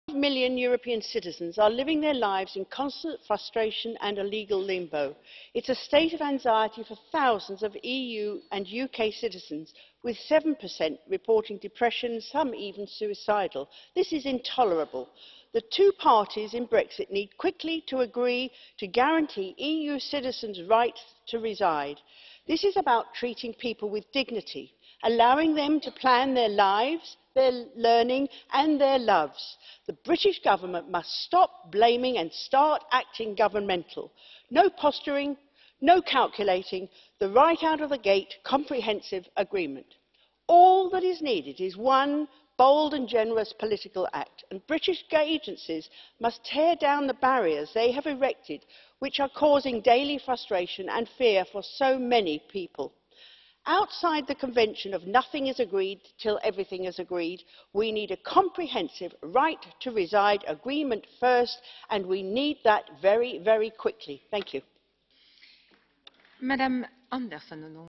EU nationals in the UK are distressed and anxious - a single act could reassure them
Catherine Bearder MEP European Parliament speech 17th May 2017:

5m European citizens are living their lives in constant frustration and a legal limbo.

There is a state of anxiety for 1000s of EU and UK citizens with 7% reporting depression and some even suicidal.

This is intolerable!

The 2 parties in Brexit need to quickly agree to guarantee EU citizens’ Right to Reside.

This is about treating people with dignity, allowing them plan their learning,
their loves
and their lives.

The British government must stop blaming and start acting governmental.

No posturing, no calculating, a “right out of the gate” comprehensive agreement.
All that is needed is one bold and generous political act.

British agencies must tear down the barriers they have erected which are causing daily frustration and fear for so many people.

Outside the convention of ‘Nothing is agreed till everything is agreed’

we need a comprehensive ‘right to reside’ agreement,

Our citizens desperately need our help now.